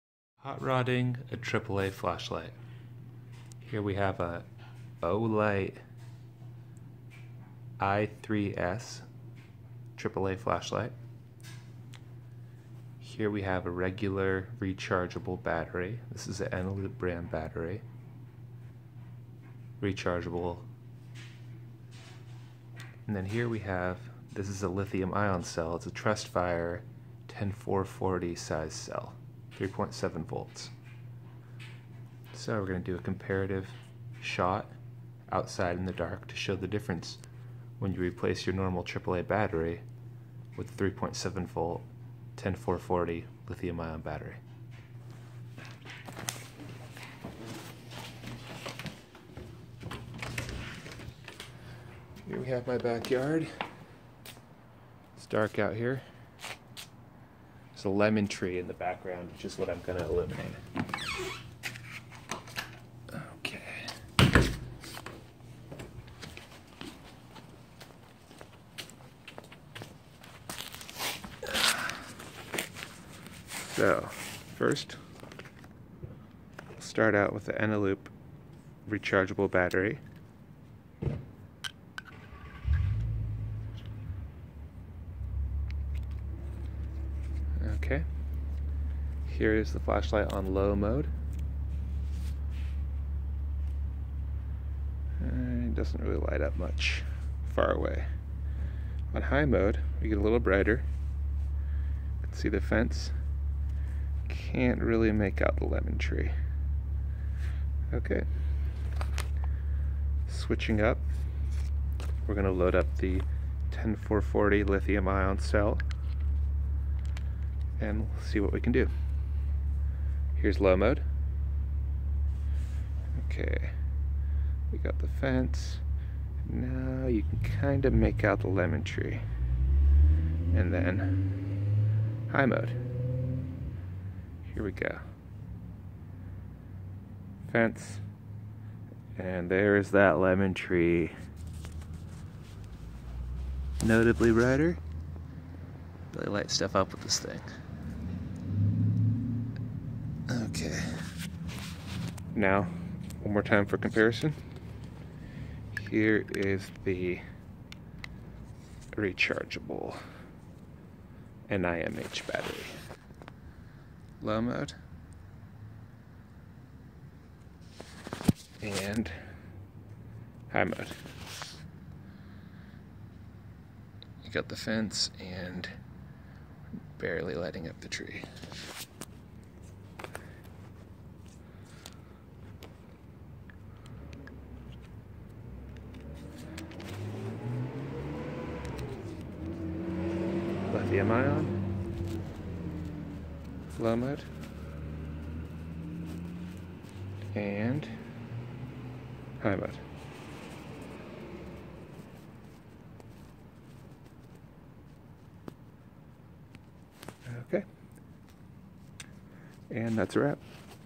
Hotrodding a 1xAAA flashlight (10440 cell)
Olight I3S w/ 10440 Lithium ion battery